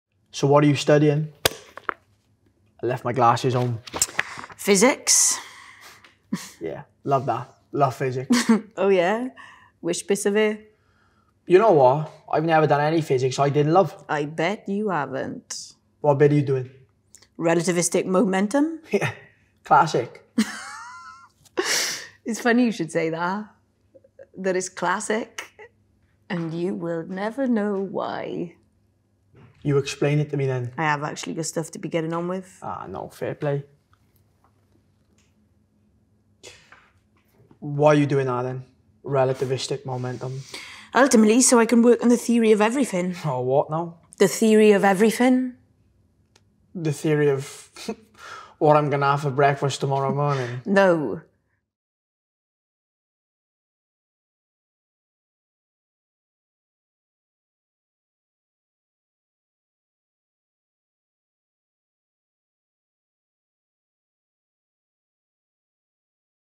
Romeo and Julie - Callum Scott Howells and Rosie Sheehy in rehearsals
Romeo and Julie
Sherman Theatre, 13 - 29 April / Theatr y Sherman, 13 - 29 Ebrill 2023
shermantheatre.co.uk/event/romeo-and-julie/
Dorfman Theatre, National Theatre 14 Feb / Chwe — 1 Apr / Ebr